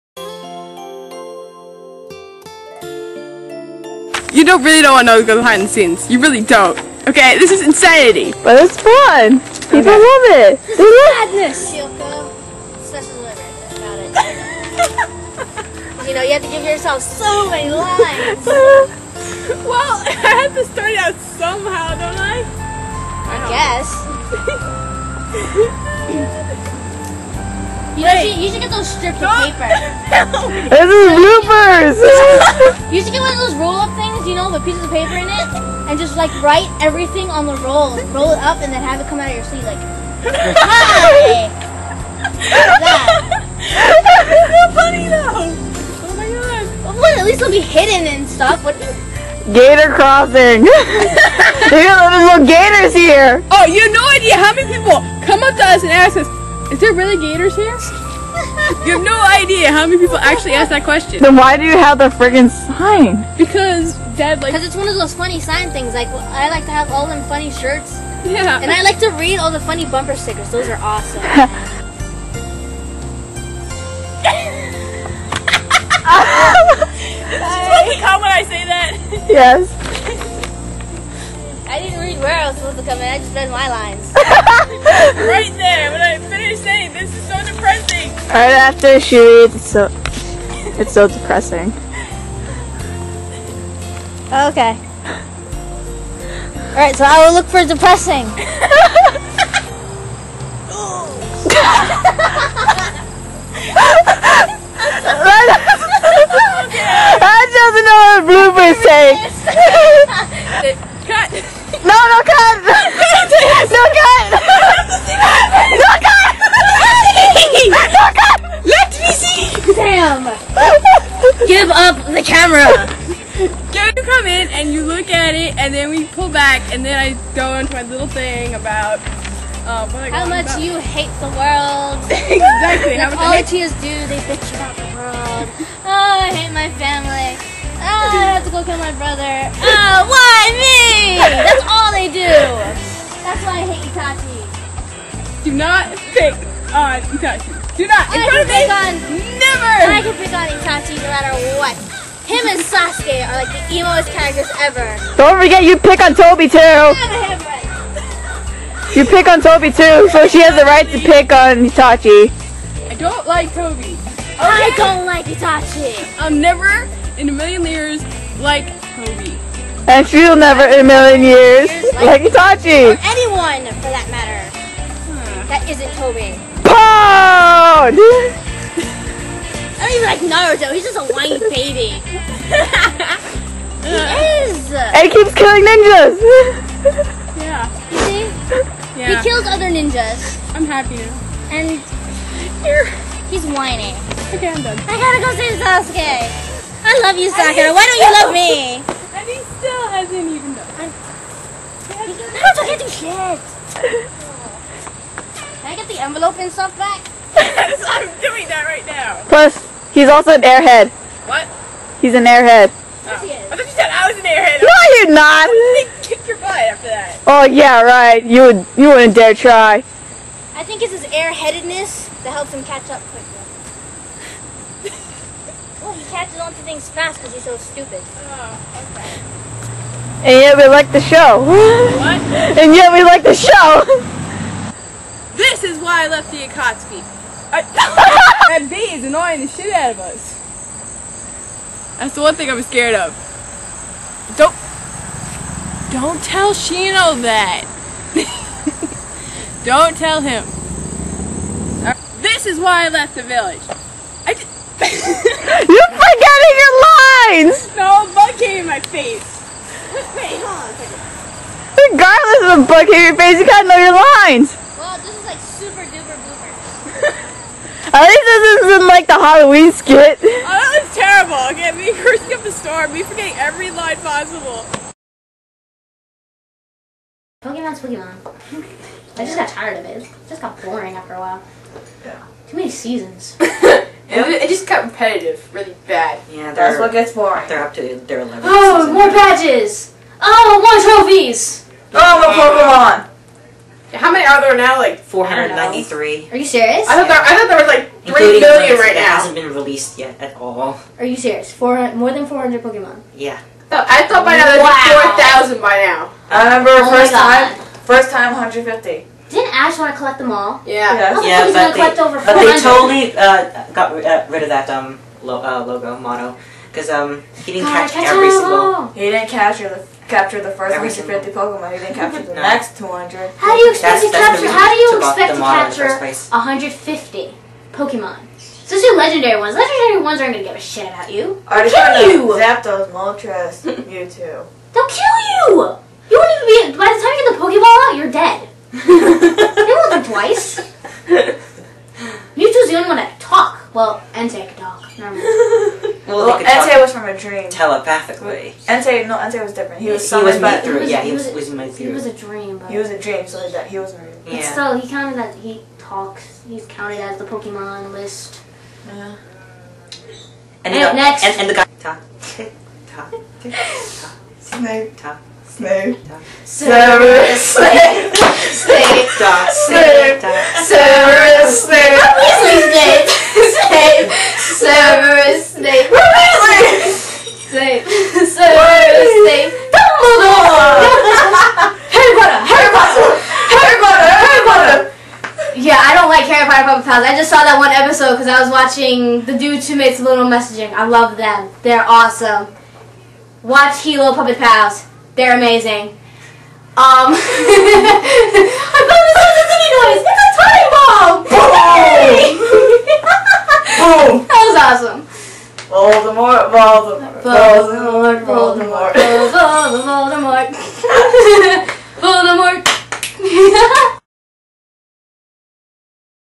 Shiuchaco Productions Outtakes
No skit can go without bloopers and outtakes. Here is a insider look to some of the behind the scenes action/comedy and outtakes that didn't make it into the original videos. (from videos: "Kioko's nightmare" and "Five fact meme")